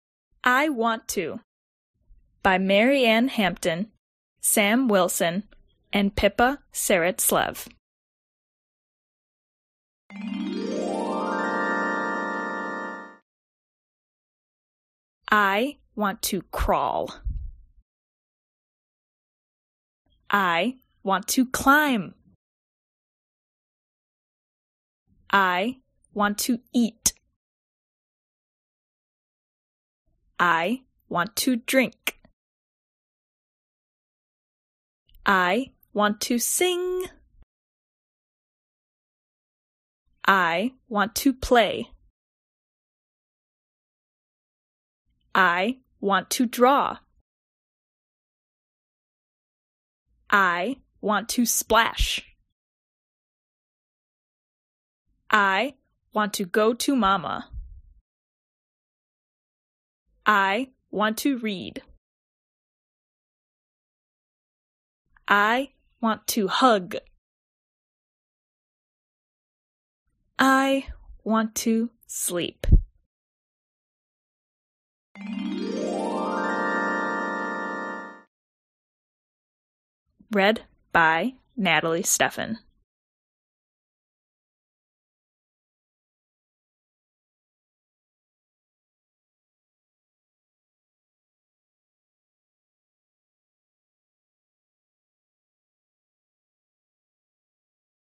[1.2] - I Want To! - (read aloud video story book @ Anglihel.com)
by
Mary-Anne Hampton (Illustrator)
Pippa Serritslev (Designer)
Robert Plummer (Editor)
Sam Wilson (Writer)

This cute little baby knows exactly what she wants. She wants to eat, she wants to play, and she wants to read a book with Mama!

Readability Level = 1.2
Read by Natalie Stephan

"I Want To!" by Mary-Anne Hampton,  Pippa Serritslev, Robert Plummer, and Sam Wilson

and is available on this YouTube channel or at Anglihel.com